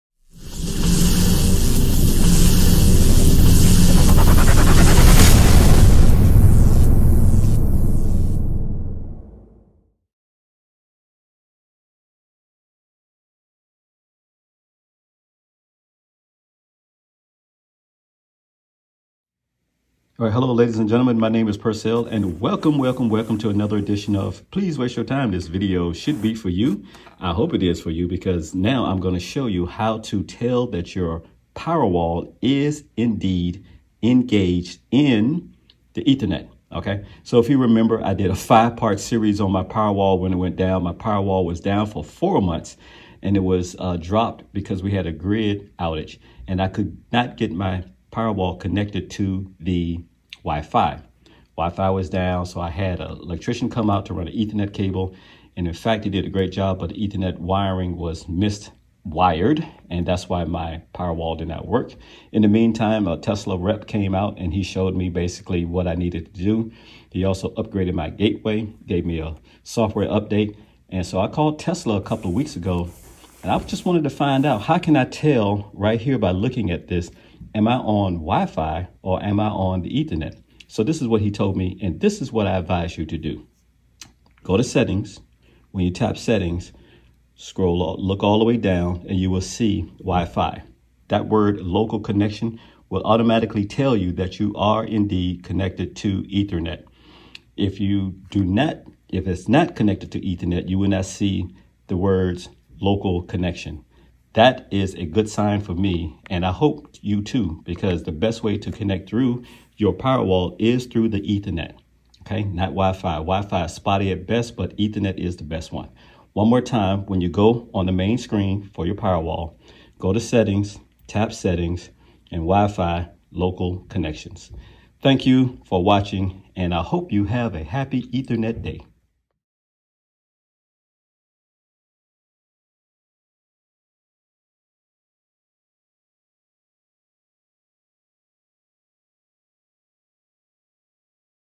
"A quick update! My Powerwall is now connected to Ethernet. How to verify on your app!
Shortly after Tesla Energy confirmed that my Powerwall was connected to Wi-Fi, I reached out to the electrician who installed my ethernet cable. He showed up the following week and confirmed (with his tester) that he indeed mis matched the end of the connector wire. Within a few minutes, it was fixed.

To find out if your Powerwall is connected to the Ethernet, allow me to show you in a quick way!